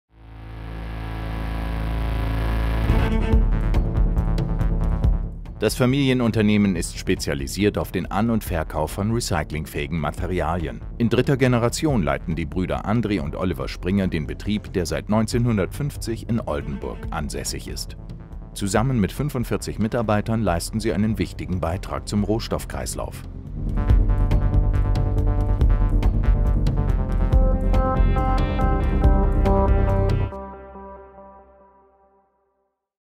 Springer und Sohn - Effizientes Recycling seit 1950
Mit 45 Mitarbeitern im Dienste des Rohstoffkreislaufes.